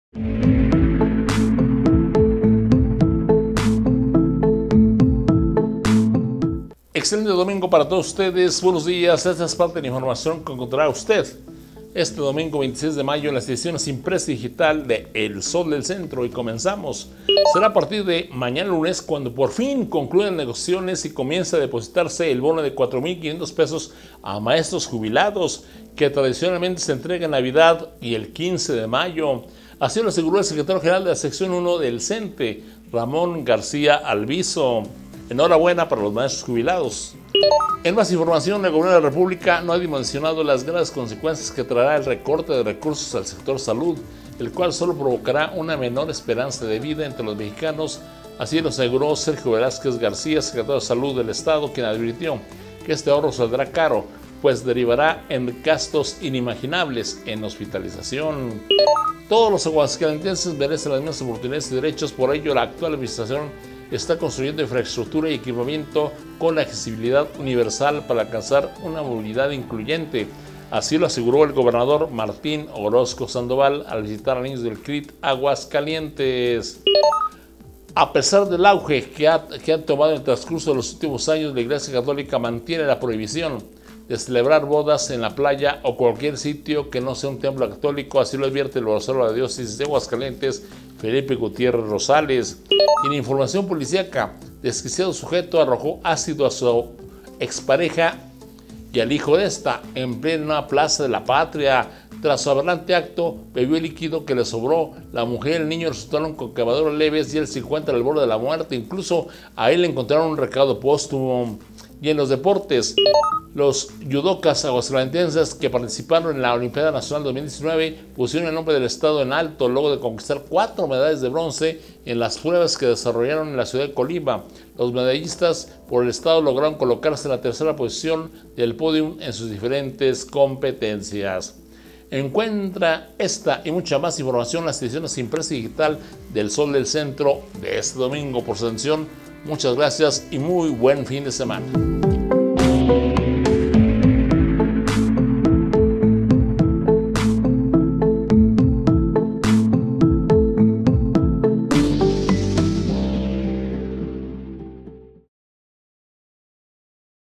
EnResumen del 26 de Mayo de 2019
Compartimos con ustedes un breve resumen con la información más relevante en el estado de Aguascalientes.